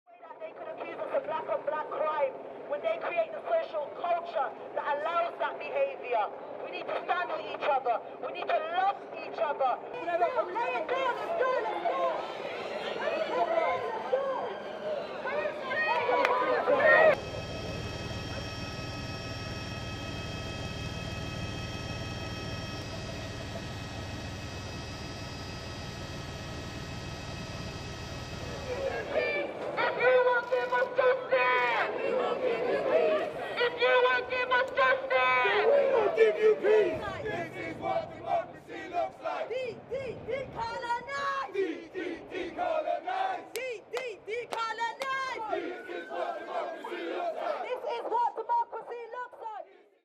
London stages 'die-in' protest against police brutality and racism
Dozens of protesters staged a "die-in" protest in central London on Sunday, against police brutality and anti-black racism.